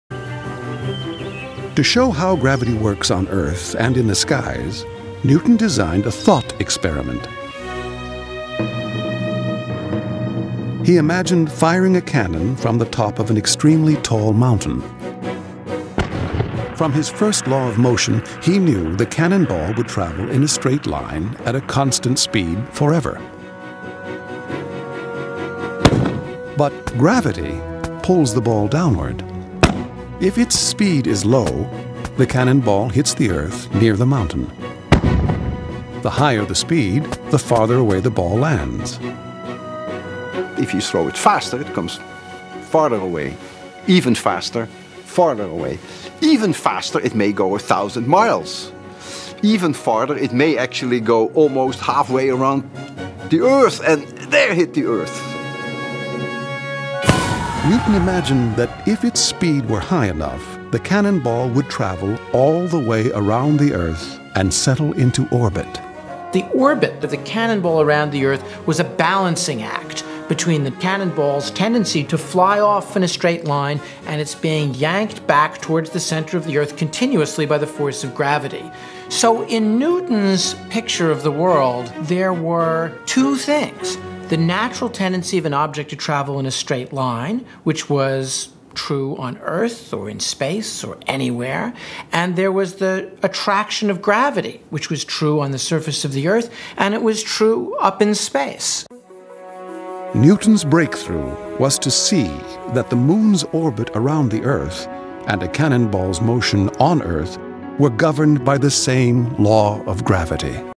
Newton - Gravitation universelle
La Gravitation universelle est régie par des lois simples. Newton en a eu l'idée à partir de la chute d'une pomme et d'une expérience de pensée.